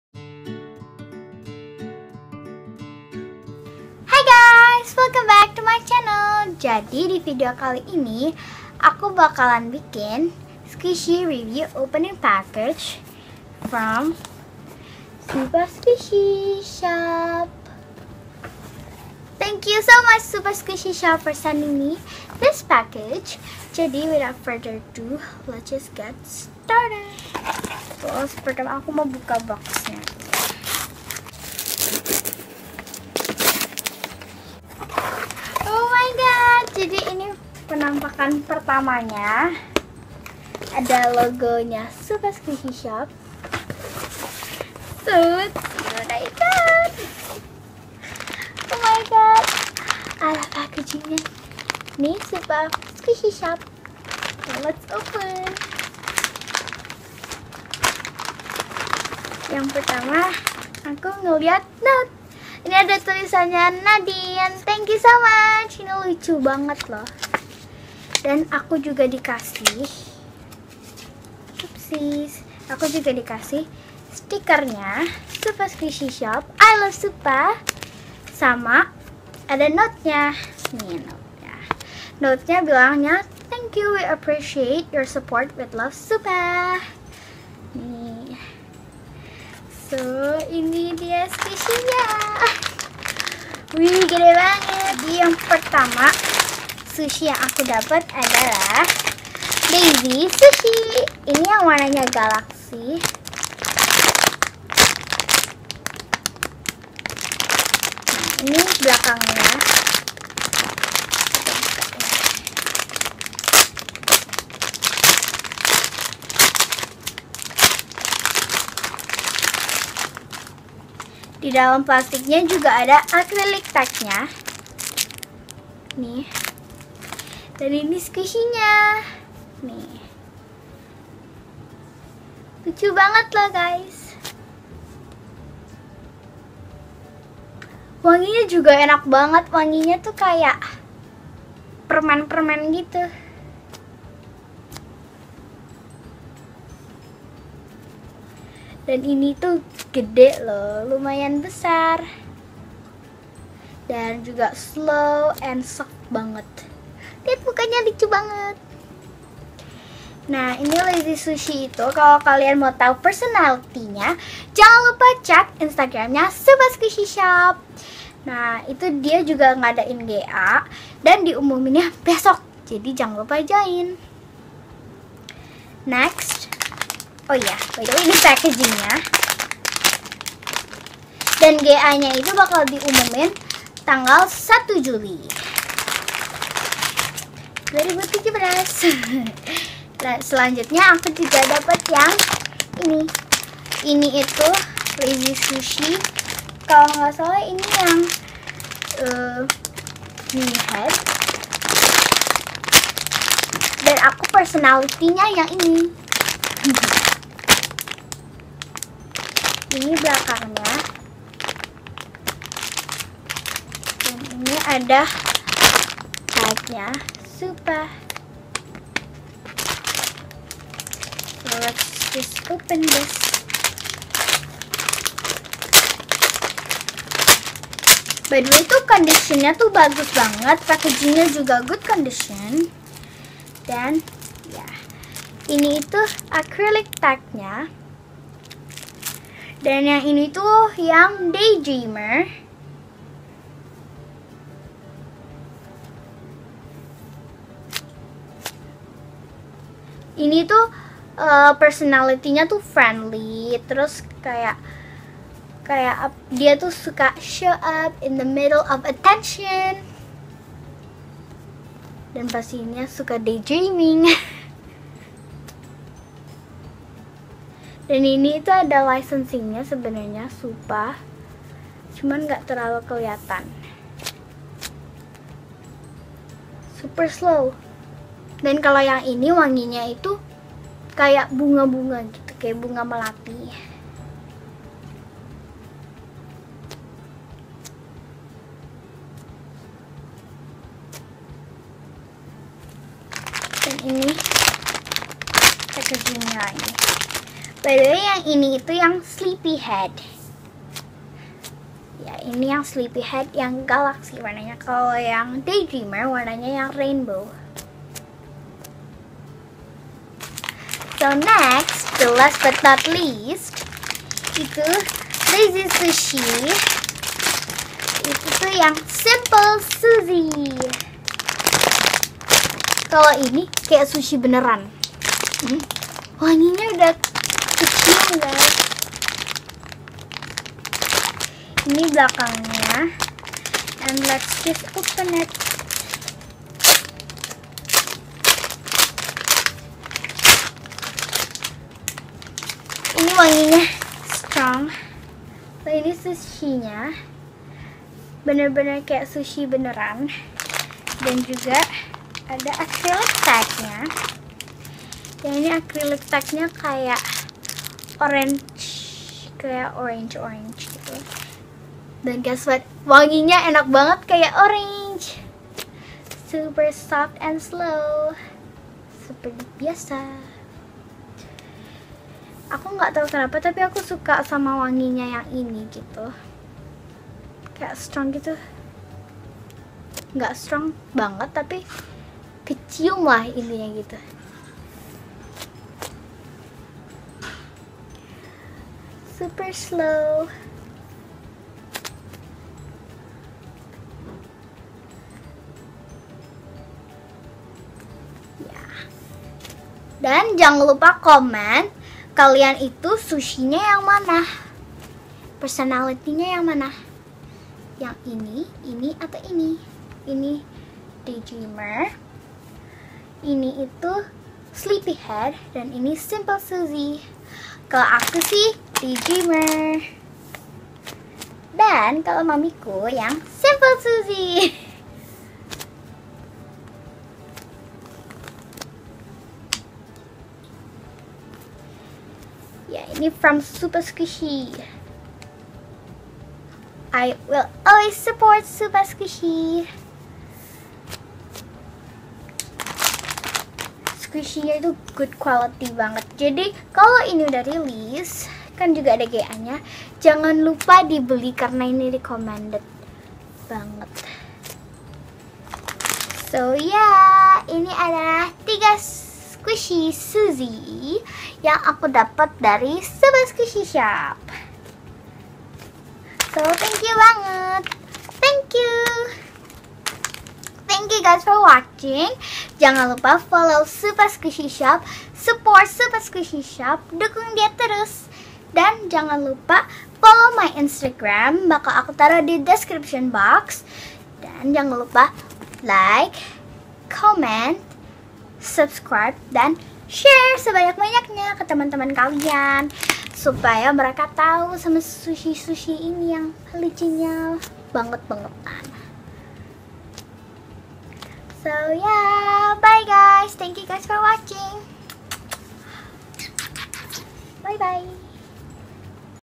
SUPASQUISHY REVIEW PACKAGE! || Nadine's Journal
Hey Guys! So aku dapet squishy package nii dari @SUPASQUISHYSHOP ON IG !! thank u bngt yaa for the review squishy package I LOVE IT SO MUCH!